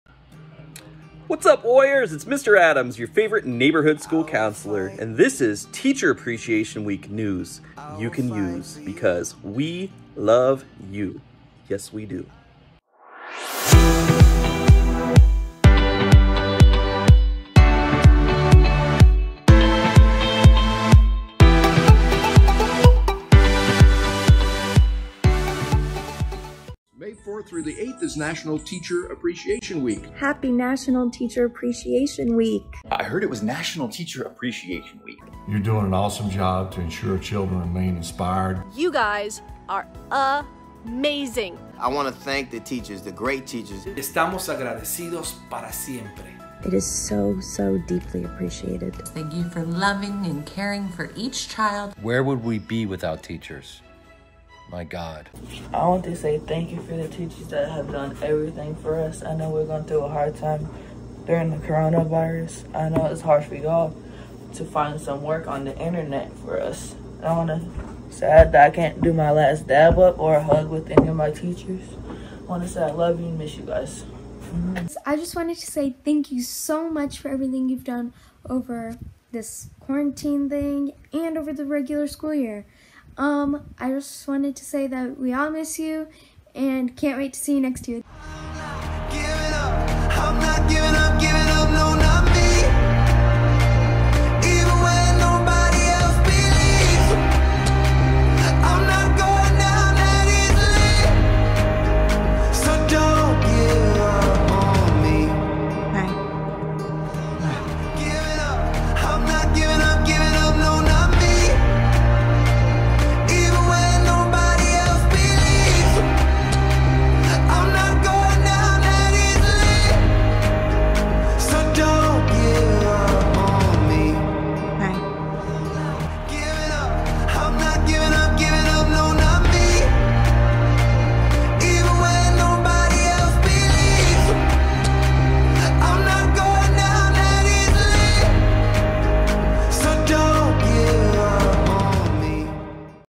Pierre Moran Middle School Teacher Appreciation Video
We wanted to recognize the amazing teachers at Pierre Moran Middle School. Therefore, with a few celebrities we borrowed from another video, some of our 7th and 8th grade students, school administrators, and school counselors, wanted to say "Thank You" for all you do!